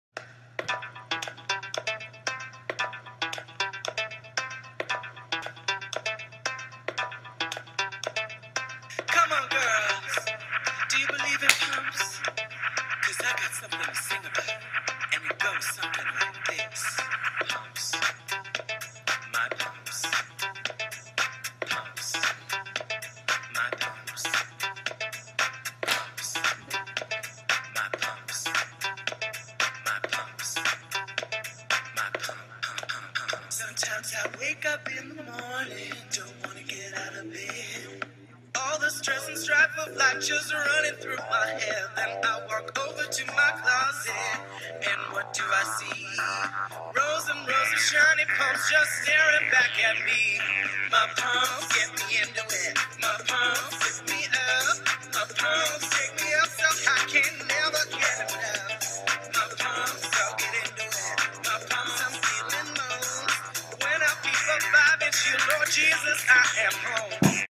MY PUMPS! spwoi7
This is our dance for an upcoming competition in LA. What do you think?? Will we win?? comment(: